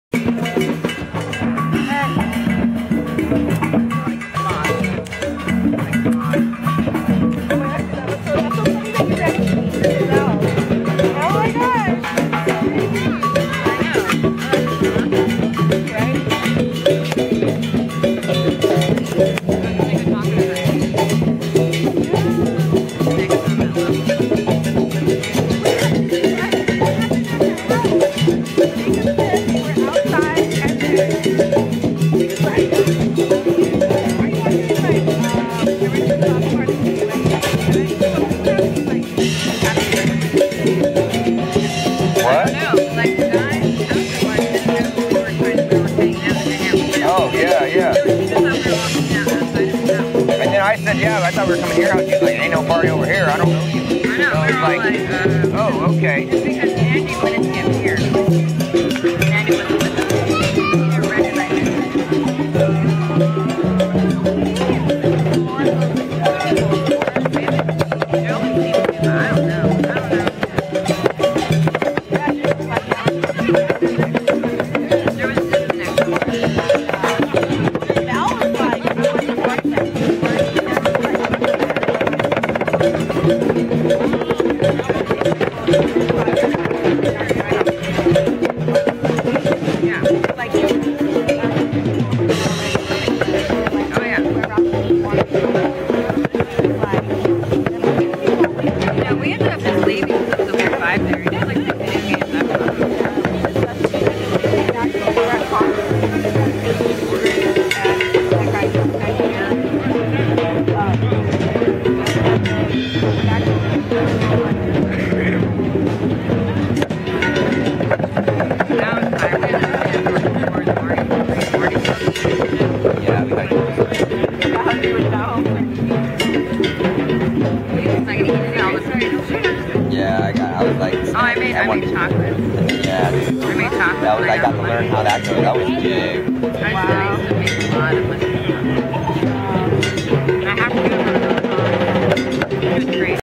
griffith park drumcircle 2014
Griffith park drum circle los angeles. Awesome drum circle been around for 40 years or so. I havent been back in a decade or so and ahh.. it feels SO GOOD!

I love drum circles. :)